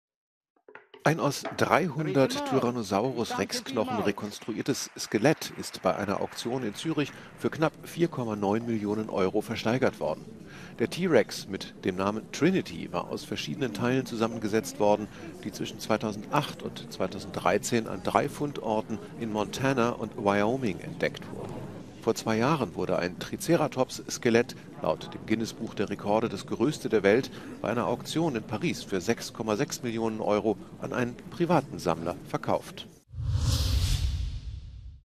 Tyrannosaurus-Rex unterm Hammer
Ein aus 300 Tyrannosaurus-Rex-Knochen rekonstruiertes Skelett ist bei einer Auktion in Zürich für knapp 4,9 Millionen Euro versteigert worden.